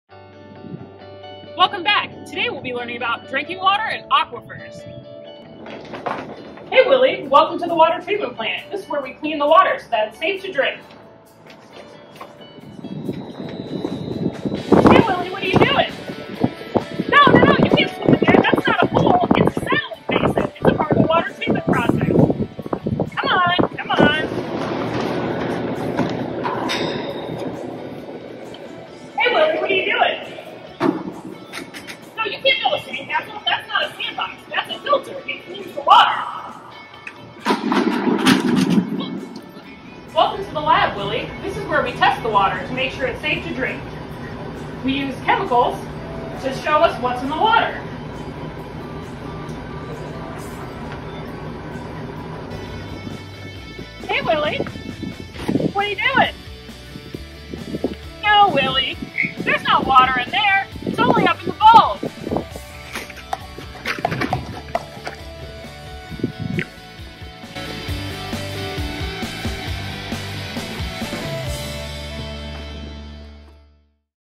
Dayton Children's Water Festival Intro - Day 3: Drinking Water and Aquifers
Willy Water visits the City of Dayton drinking water treatment plant! Do you know where our drinking water comes from in Dayton? (Hint: it's below ground...Answer: It comes from the aquifer!)